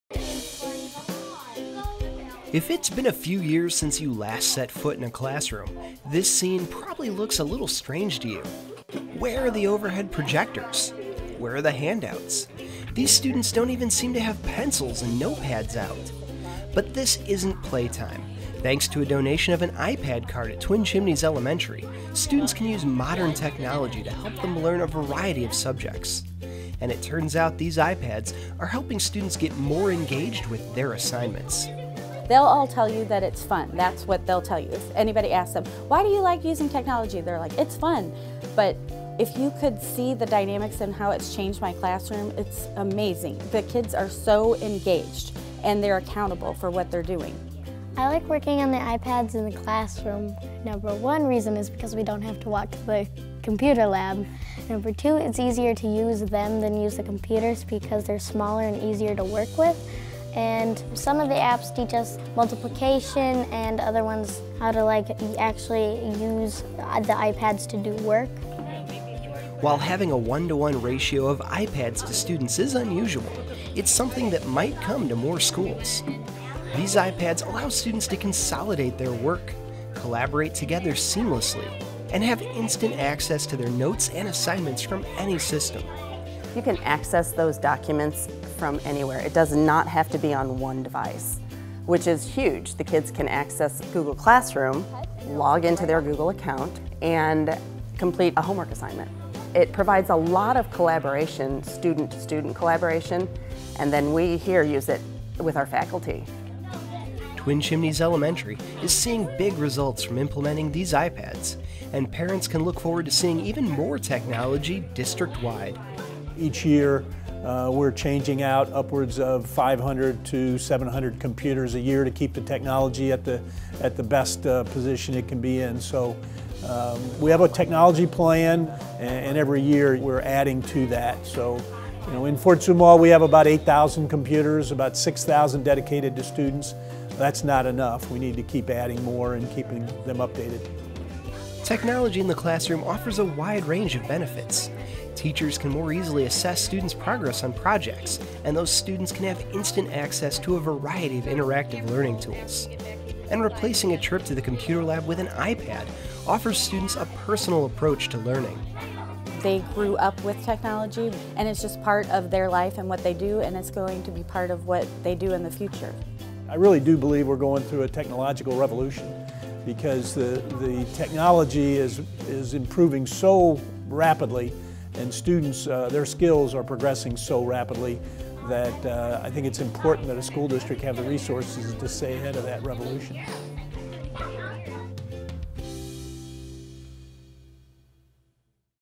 O'FallonTV: Technology in the Classroom | O'Fallon, Missouri
If it's been a few years since you last set foot in a classroom, this scene probably looks a little strange to you.  Where are the overhead projectors?  Where are the handouts?  These students don't even seem to have pencils and notepads out.  But this isn't playtime - thanks to a donation of an iPad cart at Twin Chimneys Elementary, students can use modern technology to help them learn a variety of subjects.  And it turns out these iPads are helping students get more engaged with their assignments.